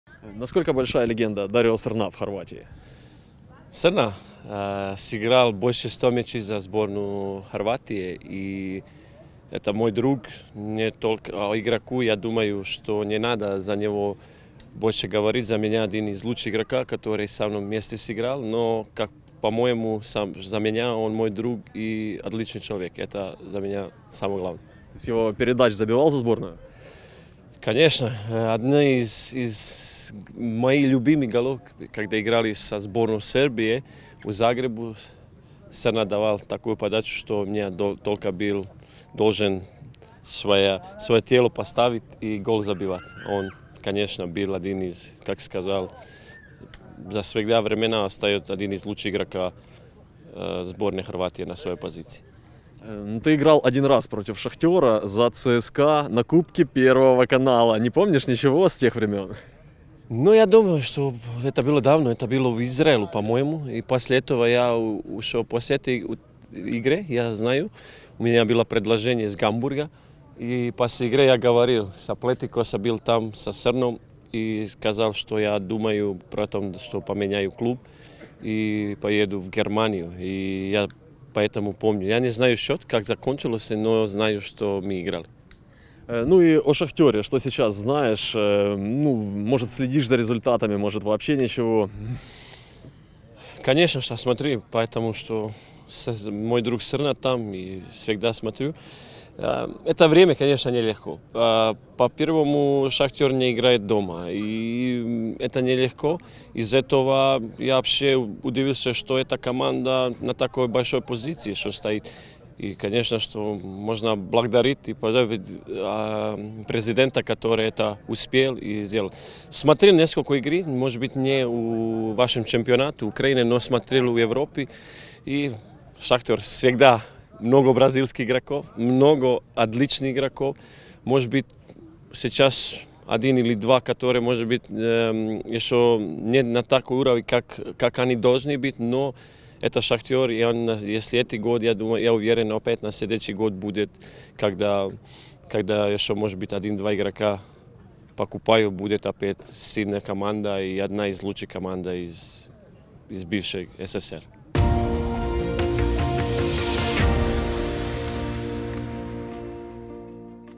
У Шахтера много отличных игроков. Ивица Олич – о Дарио Срне и горняках
Экс-нападающий сборной Хорватии Ивица Олич вспомнил матчи с Дарио Срной и рассказал, что следит за «Шахтером».

13 марта в Петрине легенды хорватского и словенского футбола встретились на поле и провели товарищескую игру в помощь региону, пострадавшему в прошлом году от землетрясения.

Все подробности поединка с участием директора по футболу ФК «Шахтер» Дарио Срны